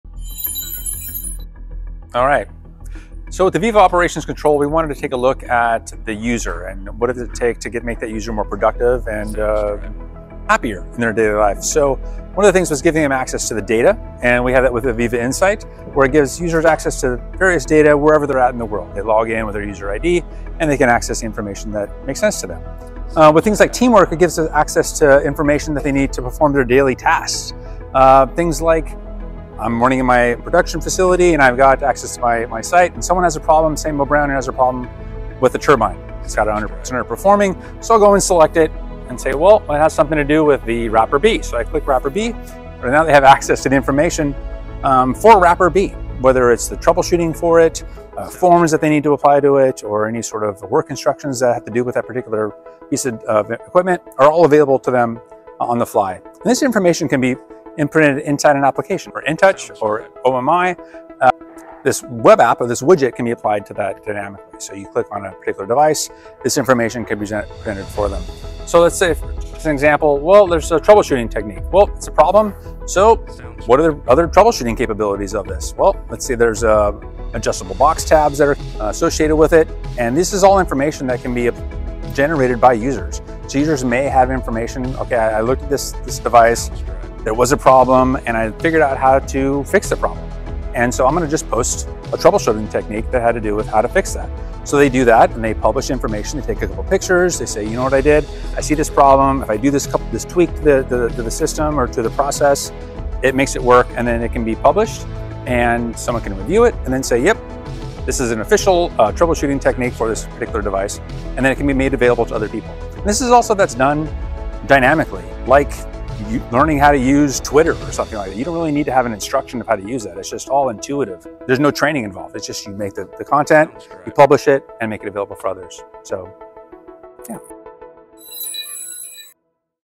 Boost Productivity & Happiness with AVEVA Operations Control!
Welcome to AVEVA Operations Control! Designed to empower technical users in their day-to-day tasks, this suite enhances productivity and satisfaction in your workflow. In this video, we explore the key features that make AVEVA Ops Control a game-changer:

🔹 AVEVA Operations Control: Developed to empower technical users, it brings efficiency and streamlined information to your daily tasks, making your work more enjoyable and productive.

🔹 AVEVA Insight: Easily access your data and make informed decisions quickly with this intuitive tool.

🔹 AVEVA Teamwork: Streamline communication and updates within your team, ensuring smoother shift handovers and improved collaboration.

The best part? All these powerful features are easy to learn and can be integrated dynamically into your existing processes.

Video by: Brian Leonard, Product Manager (AVEVA Reports for Operations & AVEVA Teamwork)